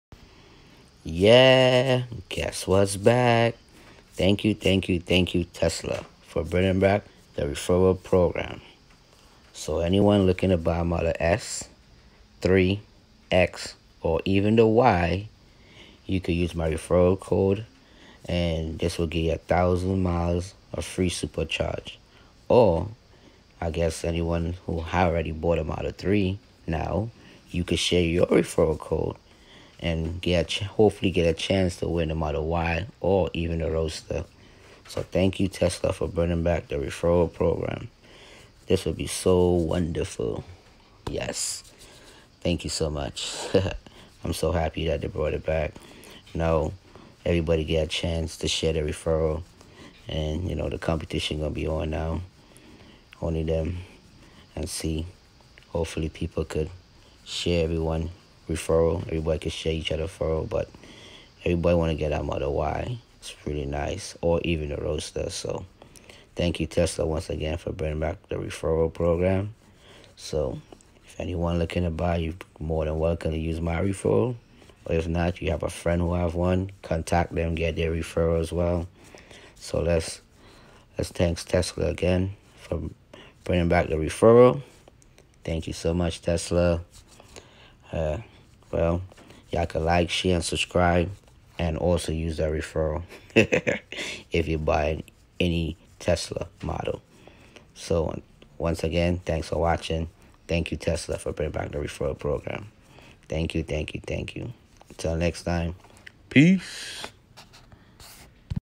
TESLA Referral program is back
Yes it’s back so if any one looking to buy a model s, model 3, model x or model y please feel free to use my referral code to get 1000 miles free

And thanks again Tesla for bringing back the referral program.